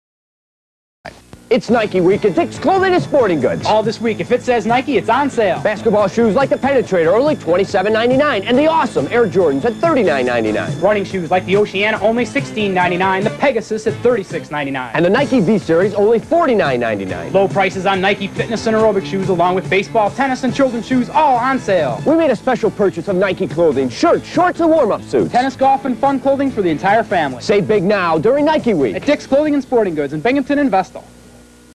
1986 Dicks Clothing & Sporting Goods "Nike Week" Binghamton Local TV Commercial
This commercial originally aired on May 5th, 1986. It was shown during an airing of an episode of the mini-series "The Deliberate Stranger" on Channel 40 WICZ.